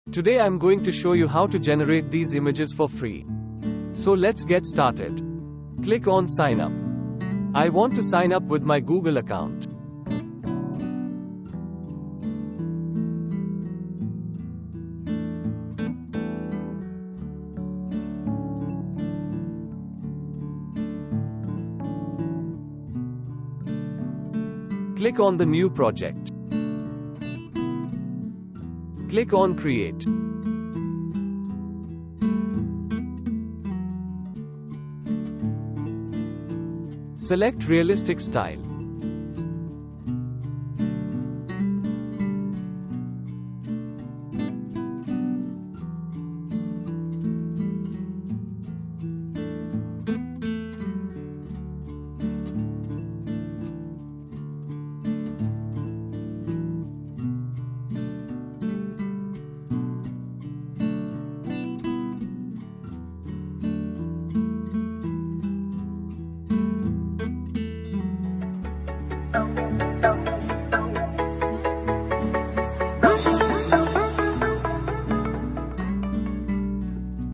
Generate this ai Trending image for free
In this educational video, we will show you how to create stunning trending letter manipulations absolutely FREE. Learn the step-by-step process and unleash your creativity with this popular design technique. From choosing the right fonts to adding effects and textures, we've got you covered. Elevate your graphic design skills and stay ahead of the trends. Don't miss out, watch now and master the art of letter manipulations!

Prompt:
3D realistic letter of  Romania Capital city, real color of buildings with letter "R" and flag.  with white background